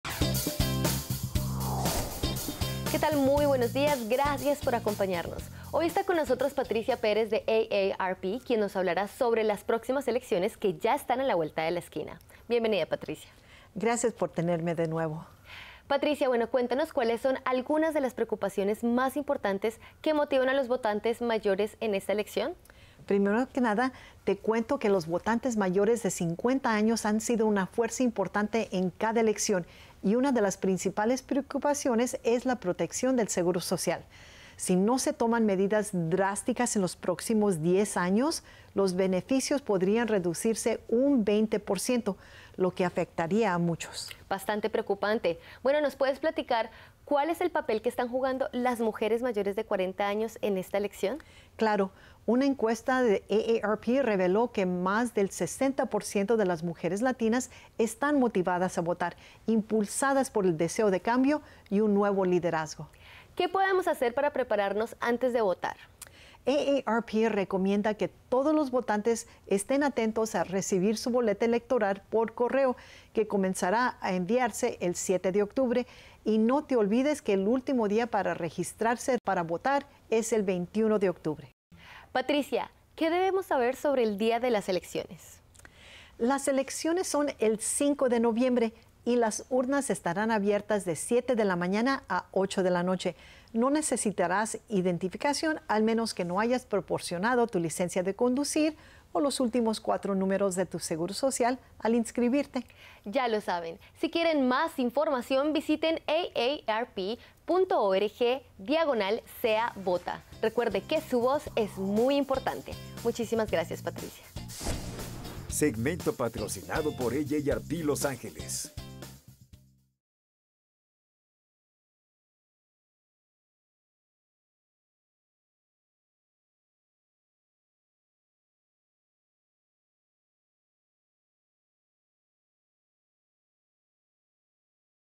AARP nos da datos importantes para votar
AARP habla sobre la importancia de proteger el seguro social y detalles importantes sobre cómo prepararse para entregar su boleta de voto.